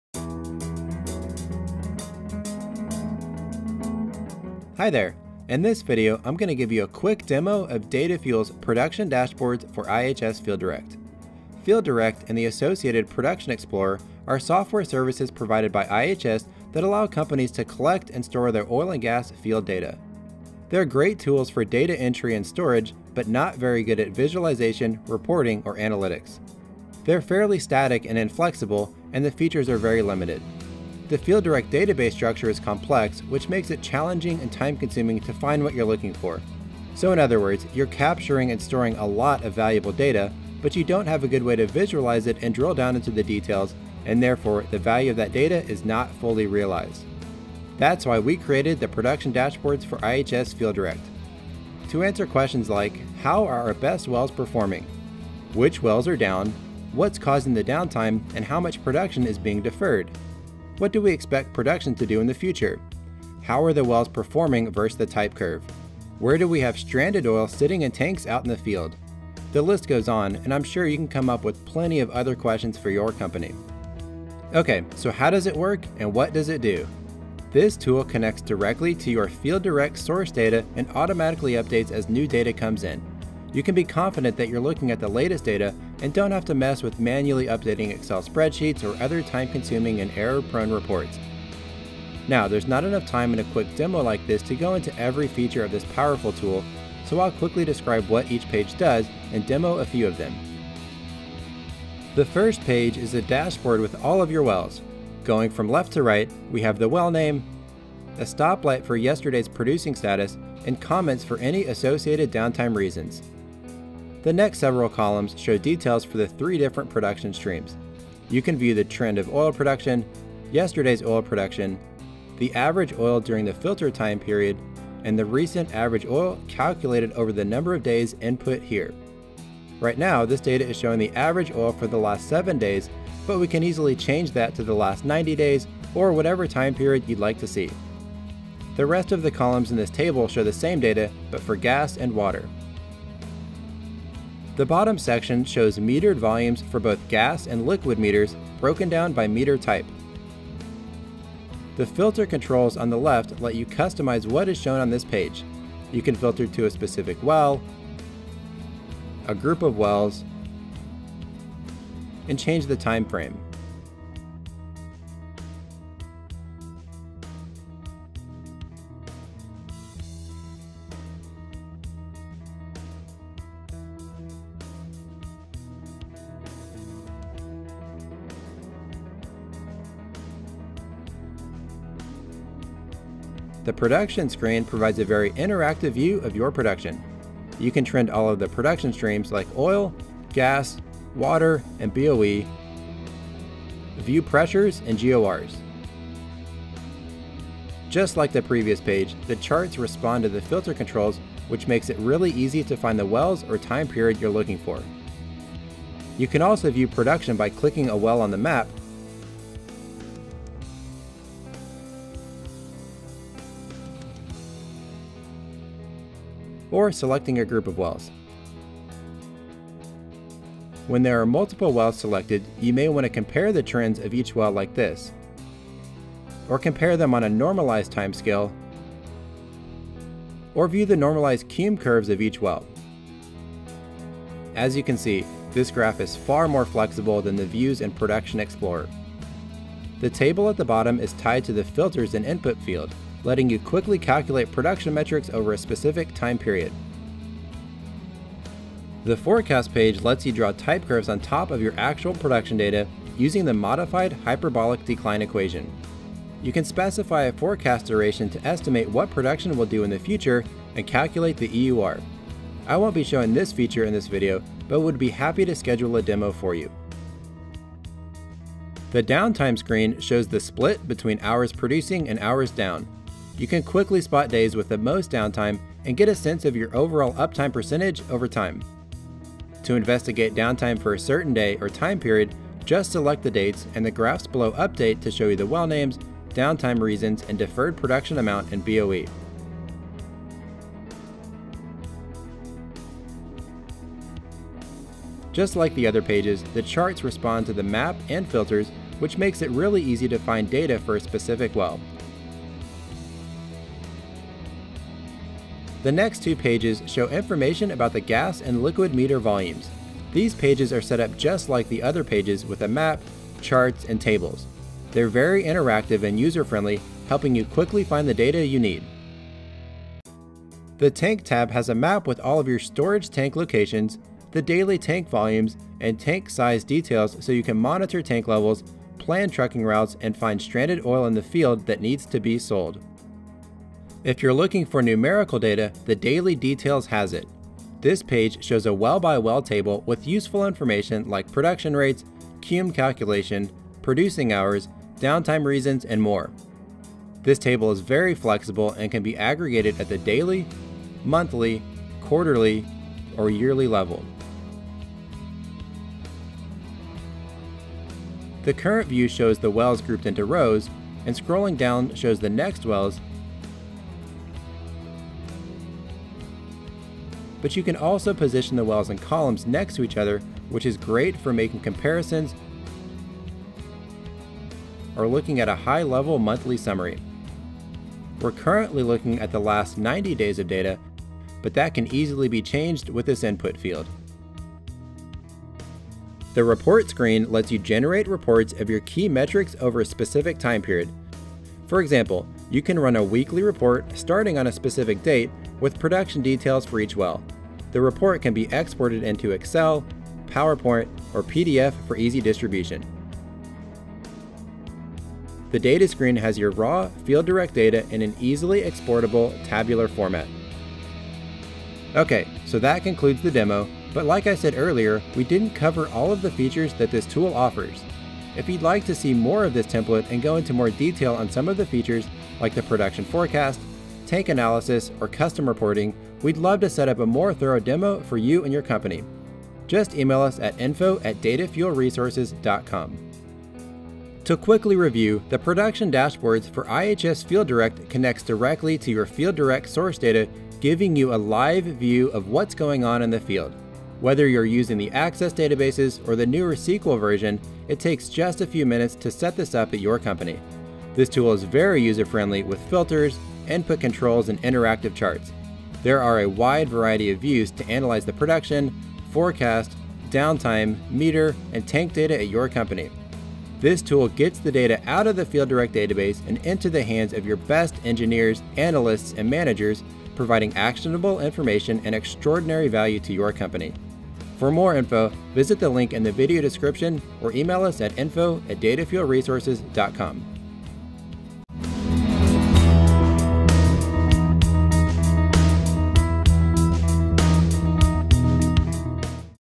Demo of Datafuel Production Dashboards for IHS FieldDIRECT
A demo of Datafuel's Production Dashboards for IHS FieldDIRECT. Take your FieldDIRECT data to the next level with actionable & insightful visual analytics. View production trends, forecast production, troubleshoot downtime, track metered and tank volumes, and much more.